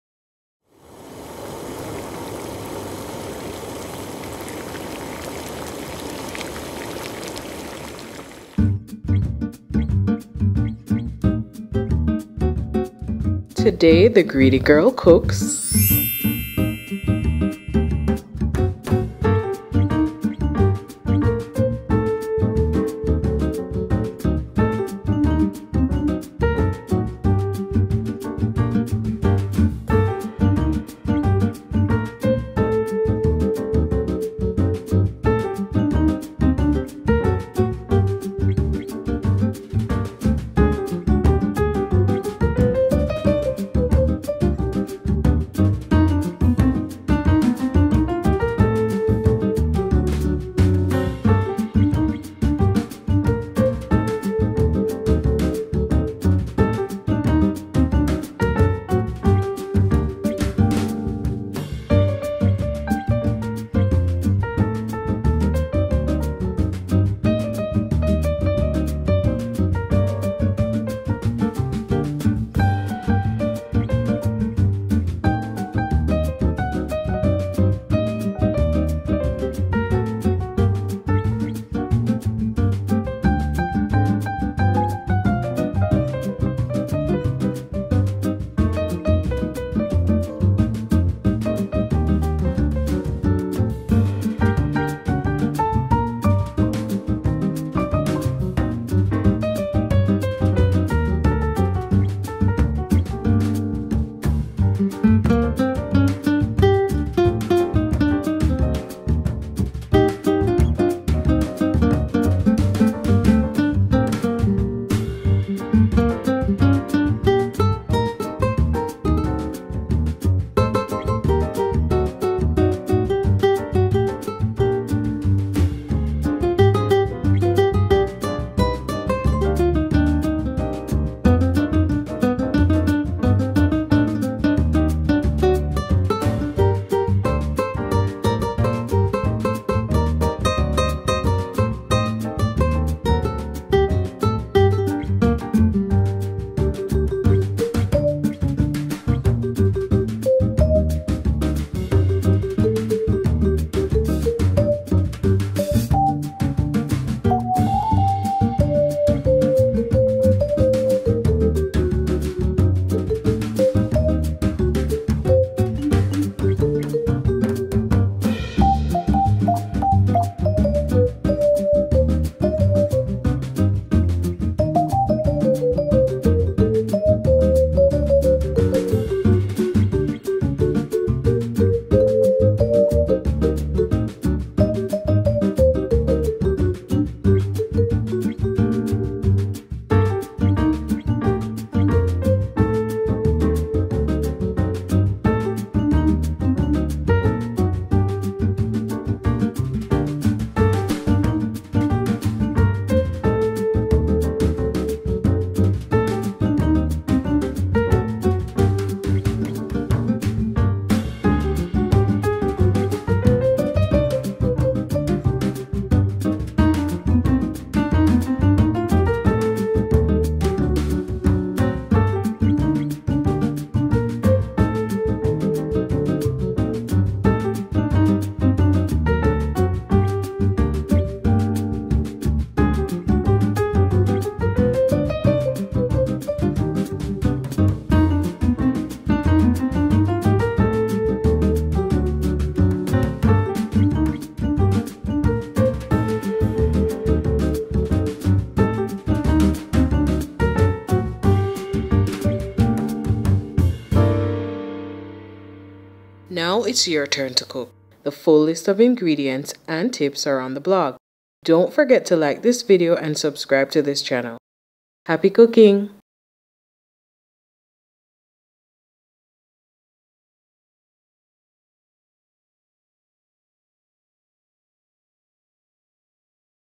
Raspberry Chocolate Ribs
Raspberry chocolate ribs, amazingly delicious Want to try it? Quick Easy and Delicious. This month its all about chocolate. non-video recipes are posted Tuesday and video recipes on Friday

check the blog for the list of Ingredients.